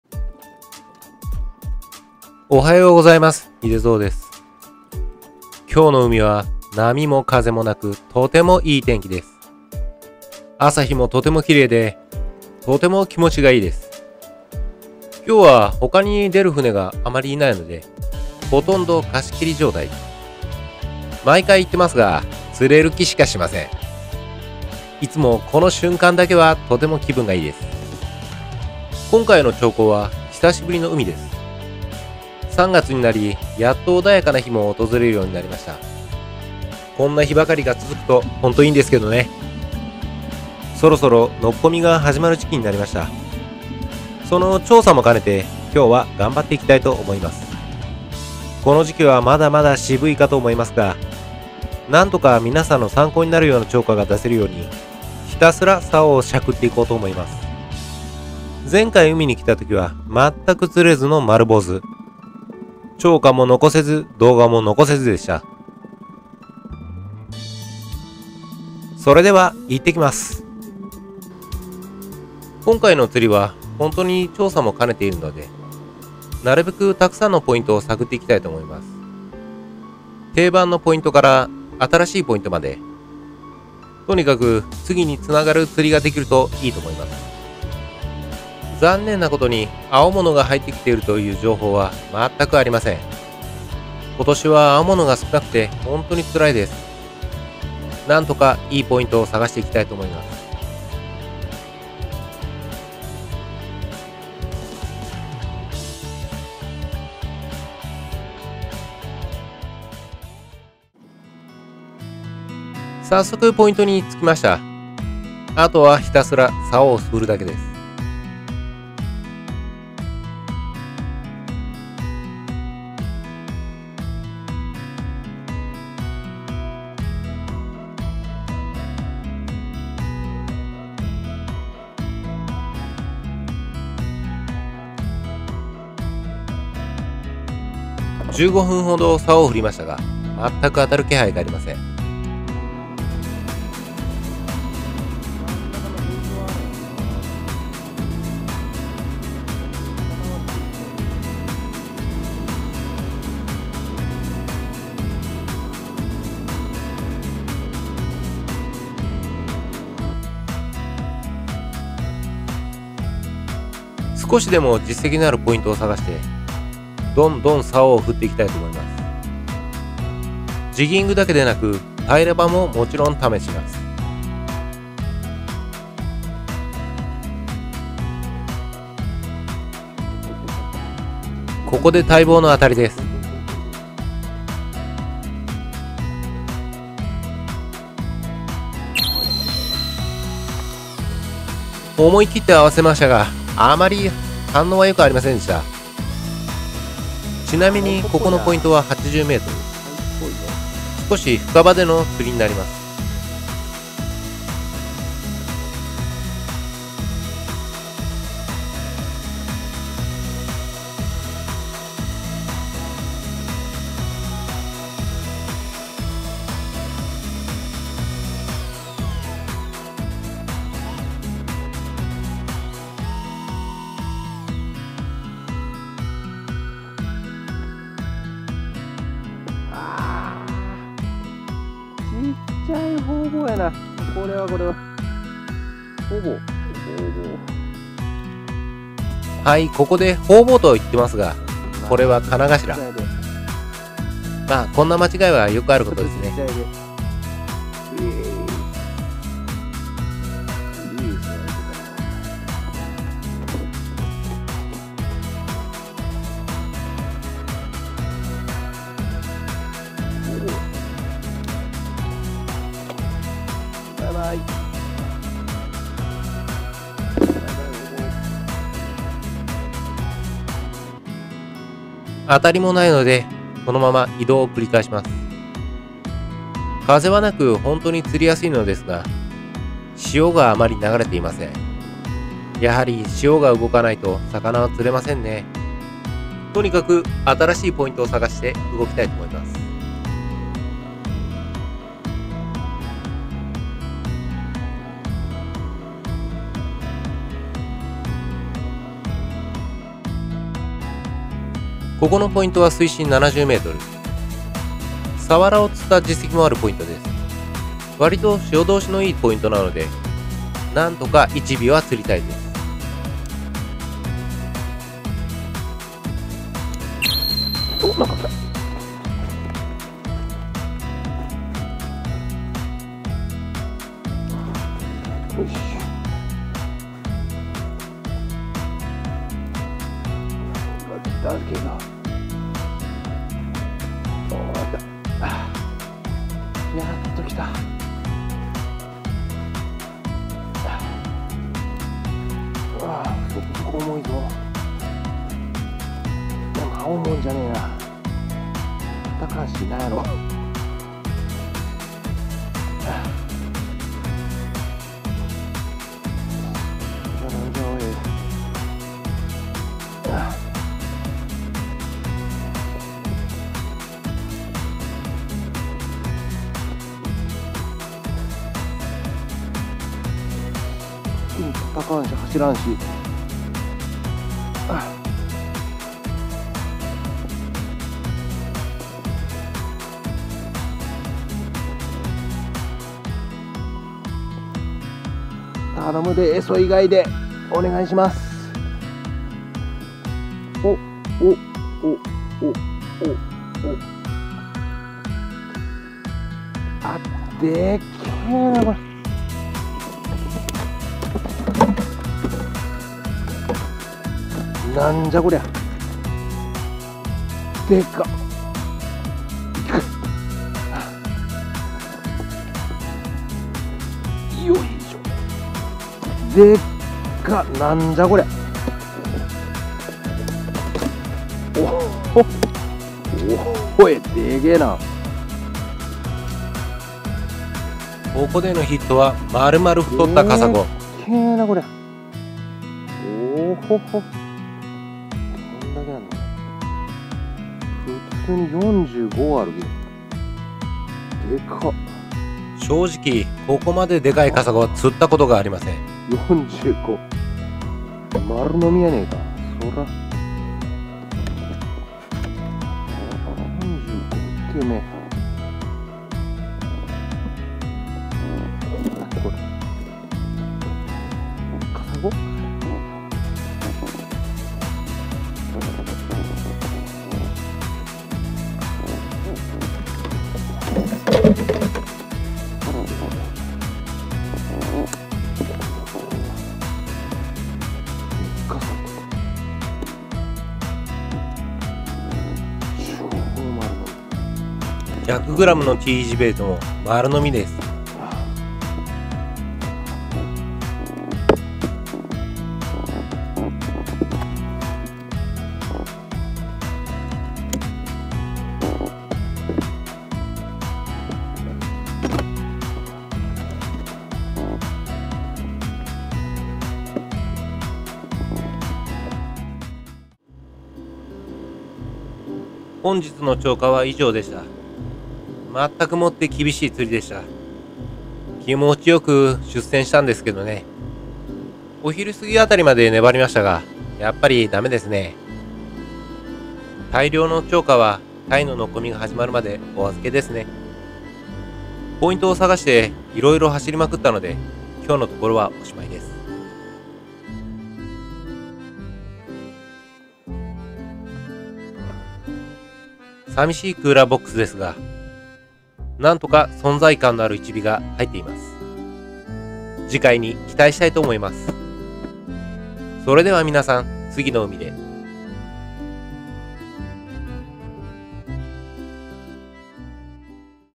2023.3 .12二馬力ボートで久々の日本海
久しぶりに日本海にやって来ました。まだまだ寒いですが今回の釣果はいかに！二馬力ボート